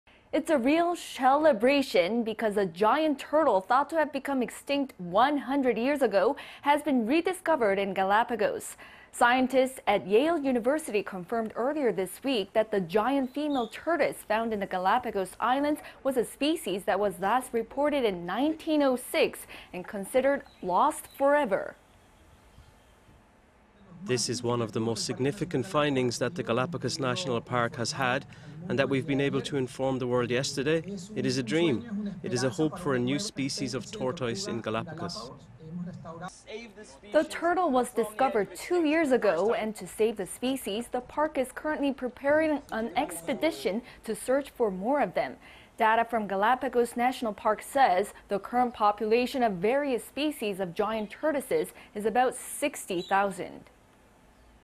Giant turtle considered extinct 100 years ago found in Galapagos Islands
갈라파고스 섬, 100년 전 멸종되었다고 믿었던 거북이 아직 살아있어

Welcome, it's time for the 'World Now'. Bringing you some of the most interesting news from around the globe, I'm Eum Ji-young.
It's a real shell-a-bration,... because a giant turtle thought to have become extinct 100 years ago has been rediscovered in Galapagos.
Scientists at Yale University confirmed earlier this week that the giant female tortoise found in the Galapagos Islands was a species that was last reported in 1906 and considered 'lost forever'.

"This is one of the most significant findings that the Galapagos National Park has had and that we have been able to inform the world yesterday. It is a dream, it is a hope for a new species of tortoise in Galapagos."

The turtle was discovered two years ago,... and to save the species, the park is currently preparing an expedition to search for more of them.
Data from the Galapagos National Park says, the current population of various species of giant tortoises is about 60-thousand.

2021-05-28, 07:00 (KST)